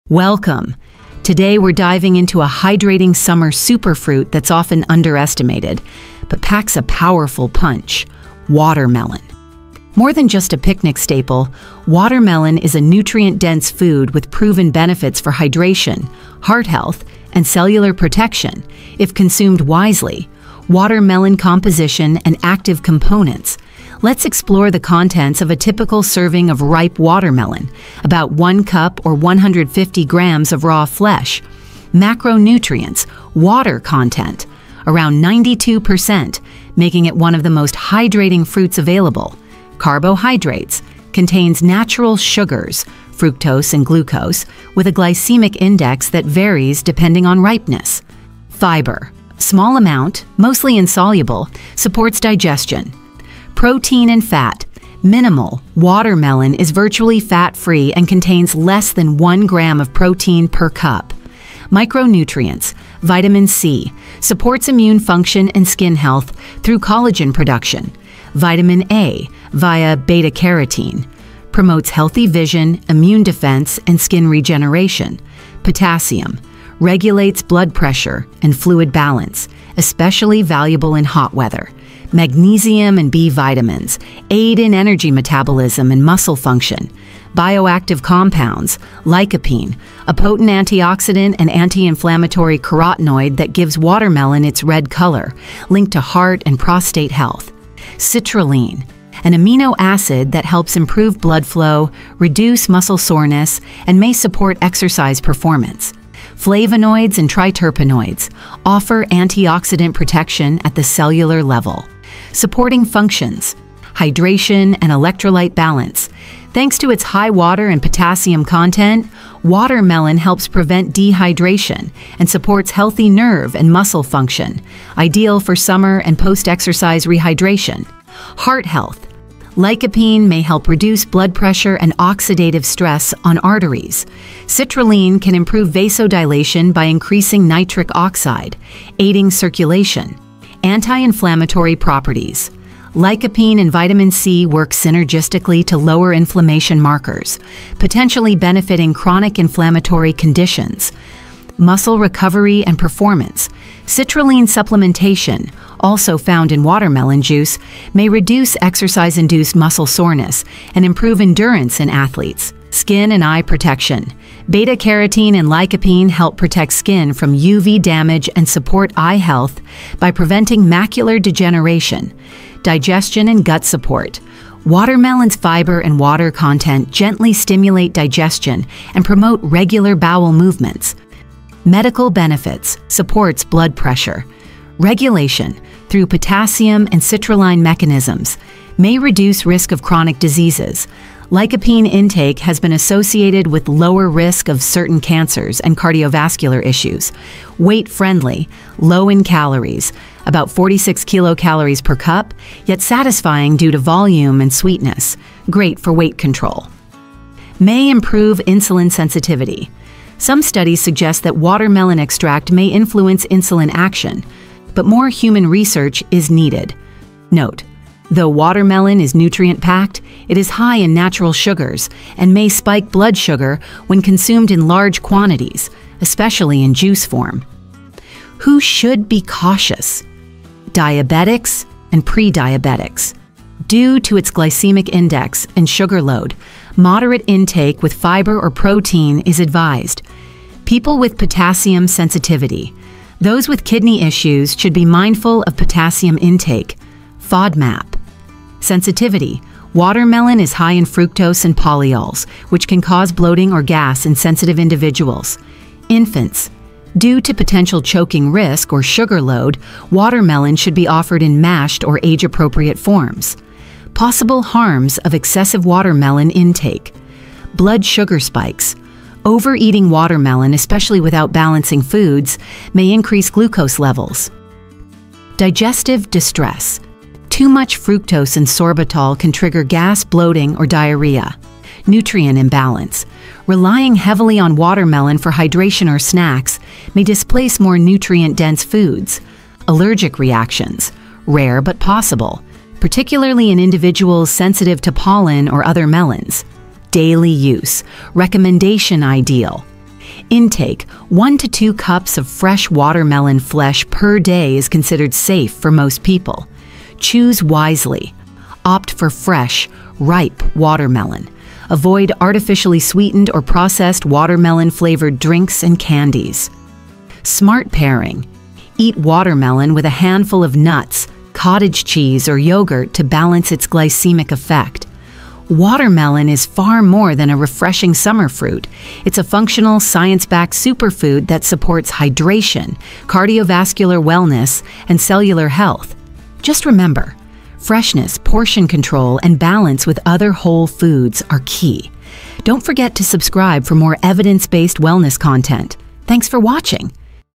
You’ll Never Look at Watermelon the Same Way | Real Medical Benefits Revealed!
Is watermelon just a refreshing summer fruit—or a powerful natural medicine?
In this video, we reveal the science-backed health benefits of watermelon, including its effects on hydration, heart health, digestion, blood pressure, and even inflammation.

Discover how lycopene and citrulline in watermelon can support your cardiovascular system and help your body recover faster after exercise. Learn about the real medical advantages of eating watermelon daily, and find out who should be cautious before consuming too much.

✅ Topics Covered:
Watermelon nutrition facts
Benefits of lycopene and citrulline
Watermelon for hydration and digestion
How it supports blood pressure and blood sugar
Possible side effects of overconsumption
Who should avoid eating too much watermelon
If you're looking to improve your health naturally, this video is for you!

⏱️ Suggested YouTube Timestamps / Video Chapters:
00:00 - Introduction: Why Watermelon Is More Than Just a Summer Fruit
00:18 -Welcome!
00:18 -Composition & Active Components
00:31 -Macronutrients
01:01 -Micronutrients
01:27 -Bioactive Compounds
01:52 -Supporting Functions
03:01 -Medical Benefits
03:37 - Note
03:48 - Who Should Be Cautious?
04:23 -Possible Harms of Excessive Watermelon Intake
04:57 - Daily Use Recommendation
05:28 -Final Thoughts: Watermelon as a Functional Superfood
05:40 -Subscribe for More Science-Based Health Tips!

 This video was prepared with the help of AI tools and is 100% original and transformed content.